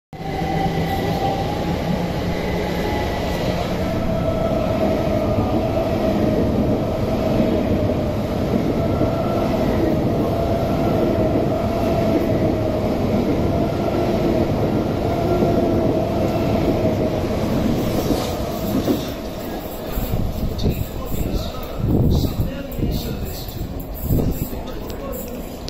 387224+227+203 pass Clapham Junction - Saturday 22nd October 2022
Govia Thameslink Railway Class 387 ‘Electrostar’ EMUs 387224+227+203 pass Clapham Junction with a London Victoria-Brighton service.